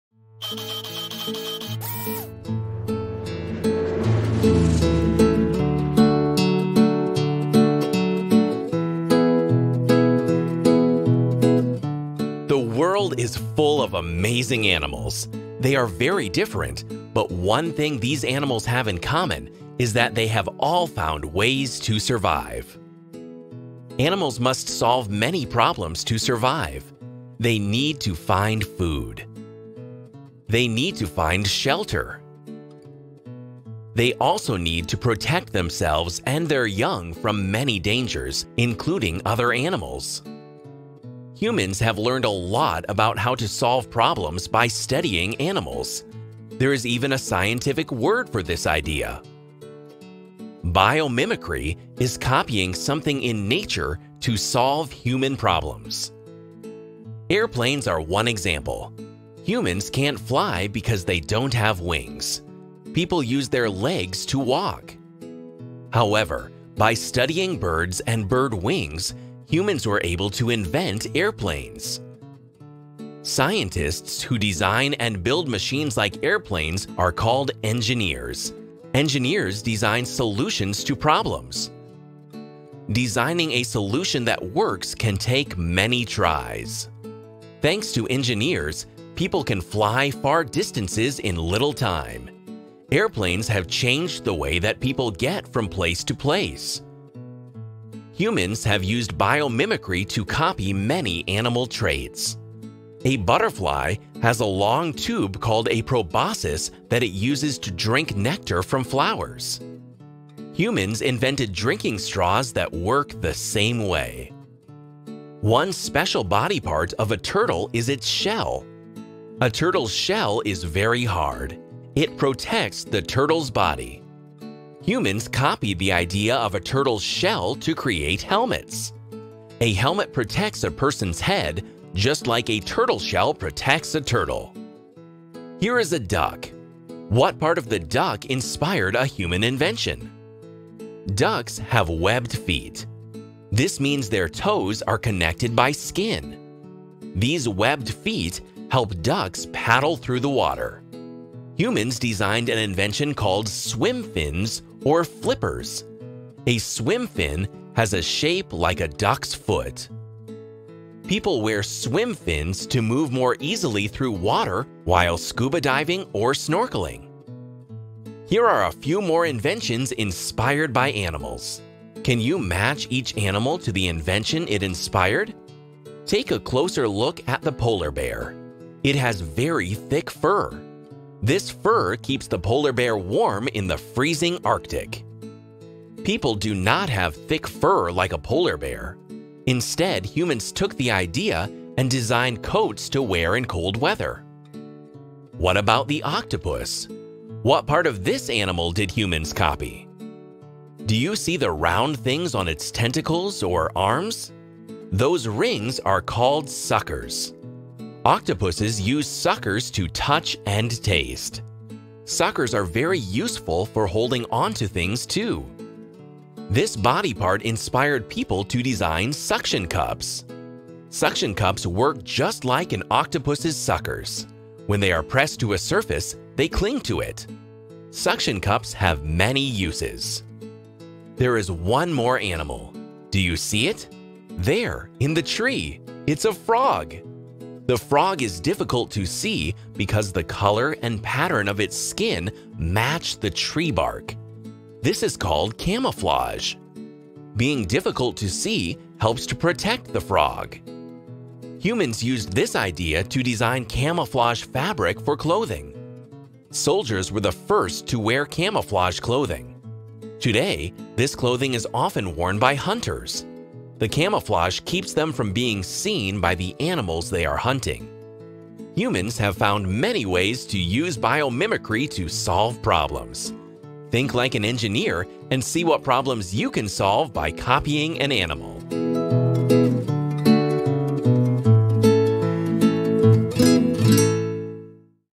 How Animals Help To Solve Human Problems
This video teaches elementary students about the ways in which scientists have studied animals in order to create solutions to problems faced by humans. For example, studying how birds fly helped scientists learn how to create airplanes. Watch the video for more examples of biomimicry, the copying of something in nature to solve human problems.